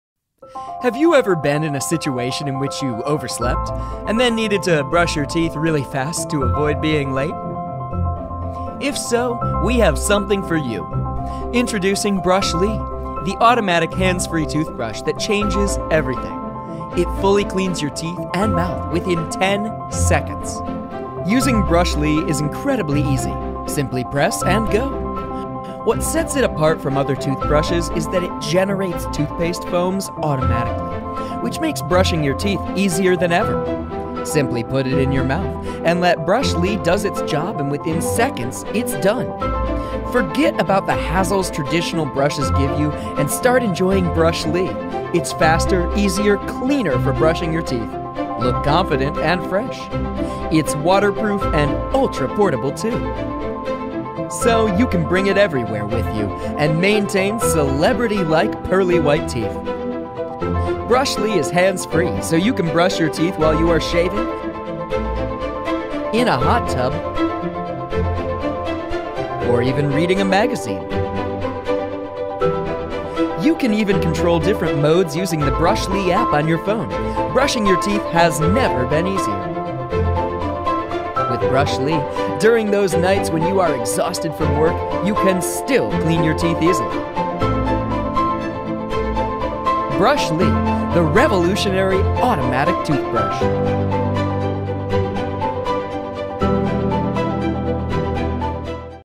Clean your teeth in just 10 SecondsBrush Lee, 10 Seconds Fully Auto-Toothbrush | Indiegogo
Clean your teeth in just 10 Seconds

Brush Lee, 10 Seconds Fully Auto-Toothbrush | Indiegogo

Brush Lee, 10 Seconds Fully Auto-Toothbrush
Auto-foaming. Clean your teeth in 10 seconds, Brush Lee makes brushing easier!

Clean the teeth in 10 seconds? It do exist. Auto-foaming, auto-brushing, no need to squeeze toothpaste, don't worry about improper brushing. Put in the mouth, press a button, wait for 10 seconds and clean done. BrushLee makes brushing easier.